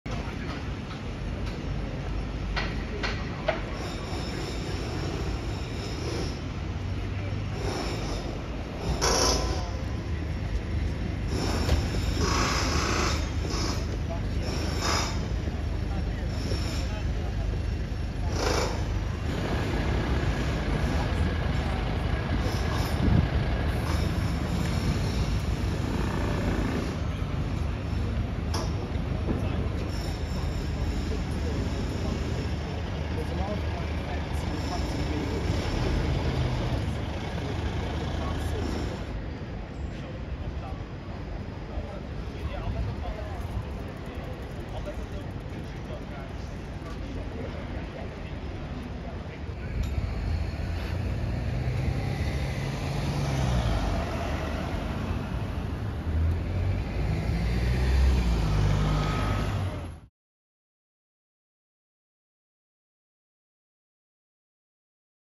Pedestrian killed by bus outside of Victoria station in London
A person has been killed after being hit by a bus outside Victoria station.

Transport for London’s chief operating officer Glynn Barton confirmed the fatality unfolded at around 9am on Monday as multiple police cars and fire engines were deployed to the scene.

Reports suggested that a Number 13 double decker bus crashed into a shelter at the busy transport hub.